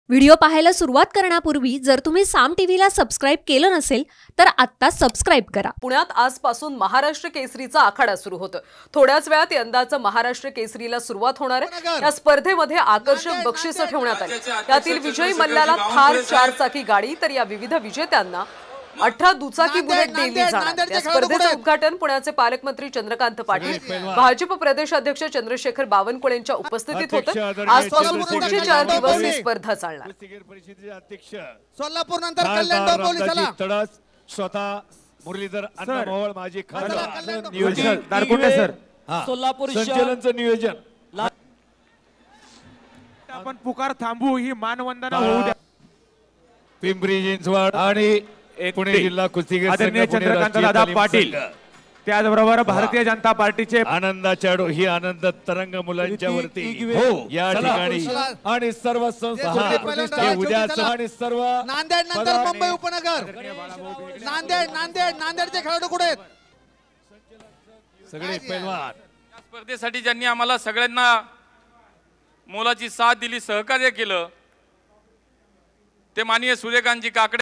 Pune : महाराष्ट्रात केसरीत थार आणि कुस्तीचा थरार, पाहा सविस्तर बातमी | SAAM TV
Pune : महाराष्ट्रात केसरीत थार आणि कुस्तीचा थरार, पाहा सविस्तर बातमी